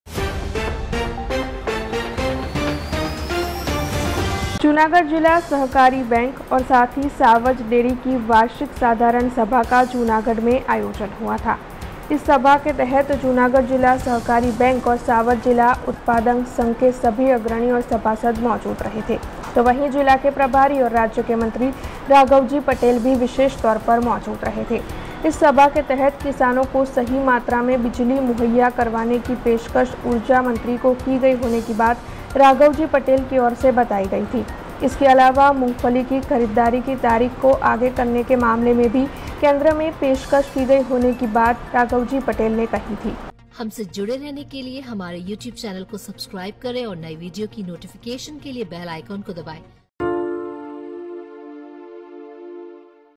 जूनागढ़ जिला सहकारी बैंक और सावज डेरी की साधारण सभा का हुआ आयोजन
General meeting of Junagadh District Cooperative Bank and Savaj Dairy organized

BRG NEWS  App: